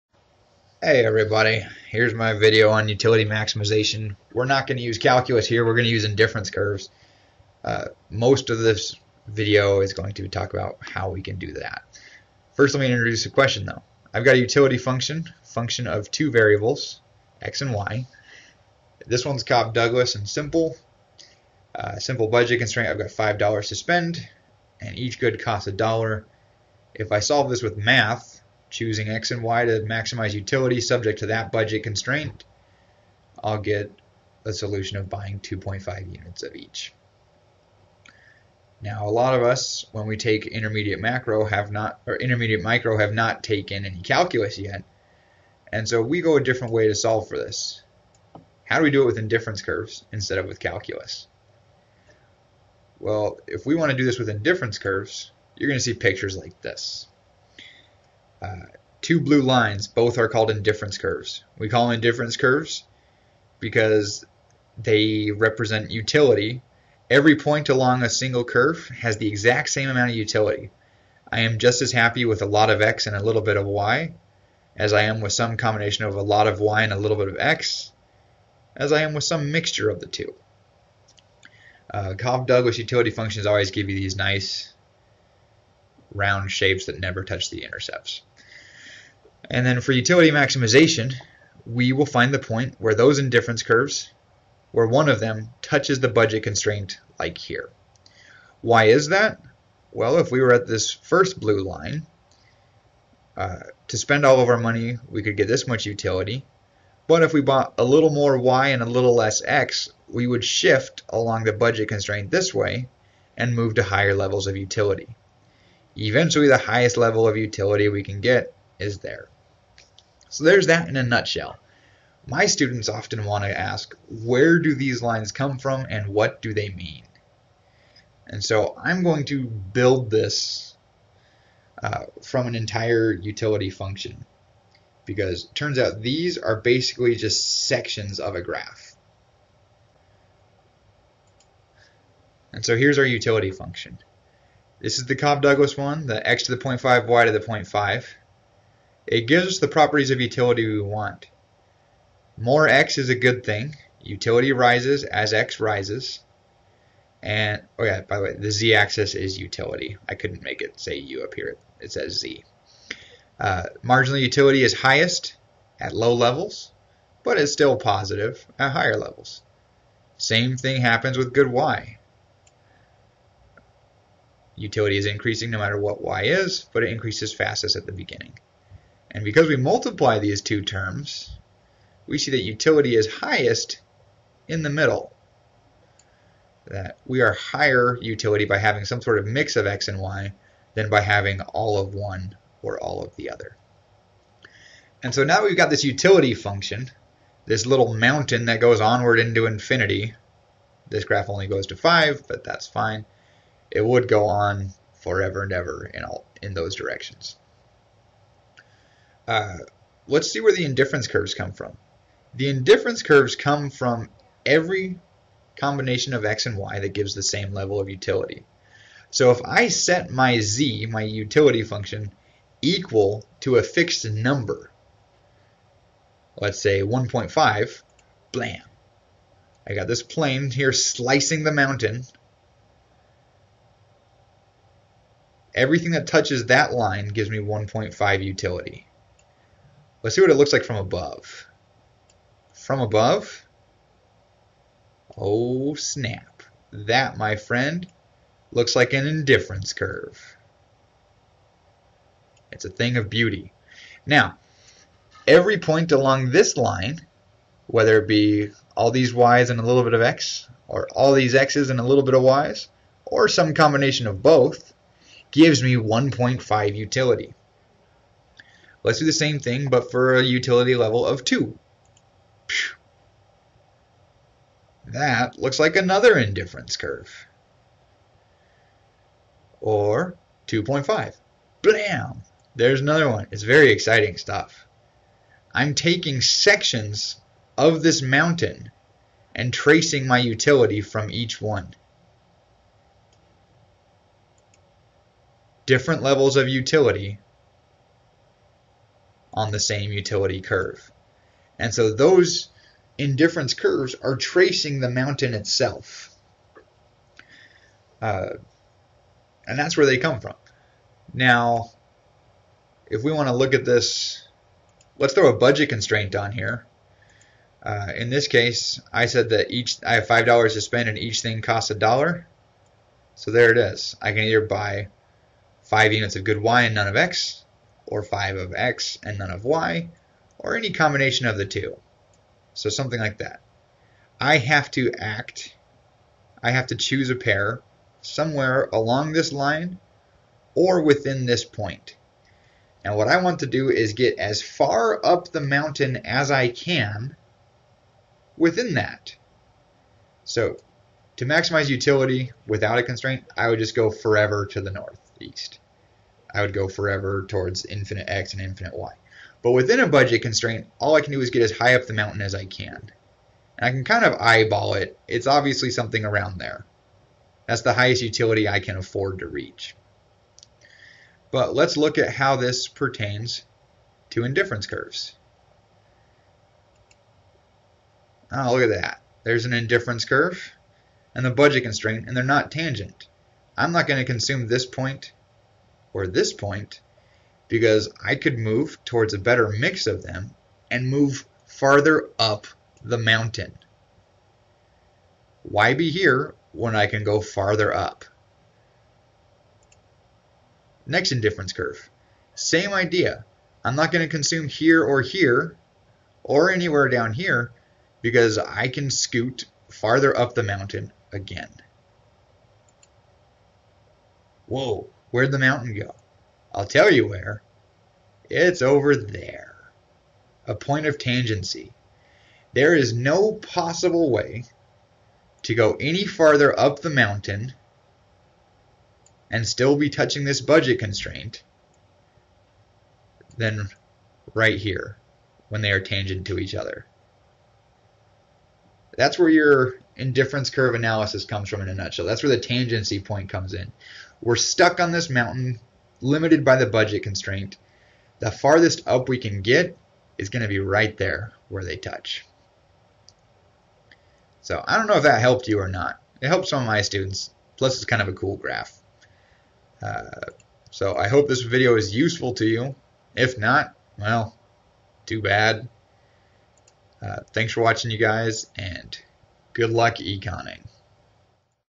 Where do Indifference Curves come from? Utility maximization with 3D pictures.
I show graphically how to maximize utility, subject to a budget constraint. I introduce the idea of indifference curves and then show where the IC's come from and why the budget constraint needs to be tangent to them. All rendered out in 3 dimensions. It is kind of nifty. It is intended to give the intuition behind the tangency of ICs and BCs.